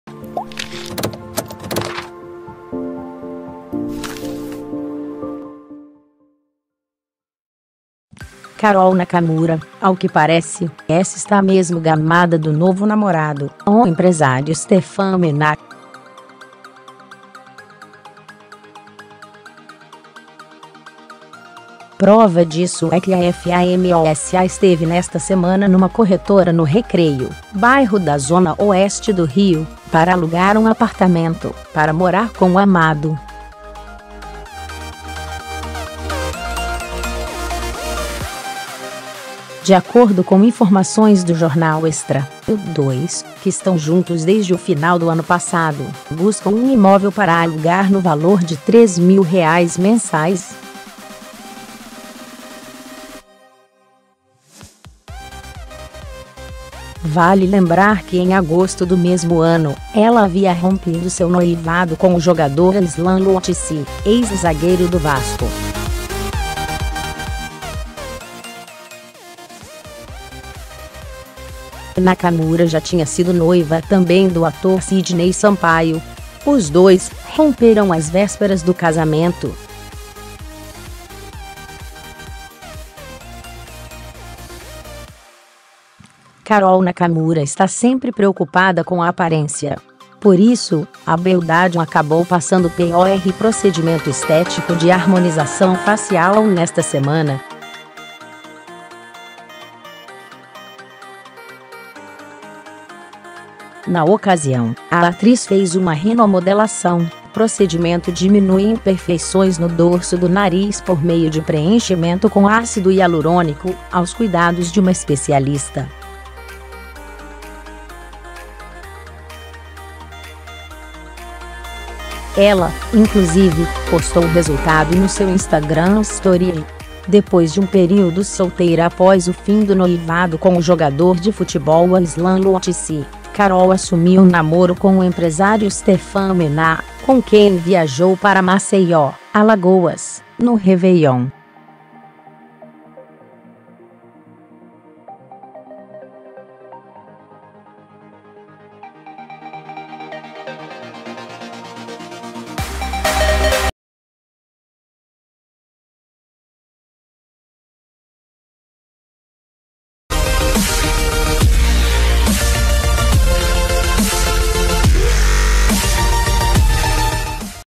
Apaixonada, Carol Nakamura vai atrás de apartamento para morar com novo namorado
Carol Nakamura, ao que parece, está mesmo gamada do novo namorado, o empresário Steffan Menah. Prova disso é que a famosa esteve nesta semana numa corretora no Recreio, bairro da Zona Oeste do Rio, para alugar um apartamento para morar com o amado.  De acordo com informações do jornal Extra, o dois, que estão juntos desde o final do ano passado, buscam um imóvel para alugar no valor de R$ 3 mil mensais.  Vale lembrar que em agosto do mesmo ano, ela havia rompido seu noivado com o jogador Aislan Lotici, ex-zagueiro do Vasco. Nakamura já tinha sido noiva também do ator Sidney Sampaio. Os dois romperam às vésperas do casamento.  Carol Nakamura está sempre preocupada com a aparência. Por isso, a beldade acabou passando por  procedimento estético de harmonização facial nesta semana.  Na ocasião, a atriz fez uma rinomodelação, procedimento diminui imperfeições no dorso do nariz por meio de preenchimento com ácido hialurônico, aos cuidados de uma especialista. Ela, inclusive, postou o resultado no seu Instagram Storie.  Depois de um período solteira após o fim do noivado com o jogador de futebol Aislan Lotici, Carol assumiu namoro com o empresário Steffan Menah, com quem viajou para Maceió (AL) no Réveillon.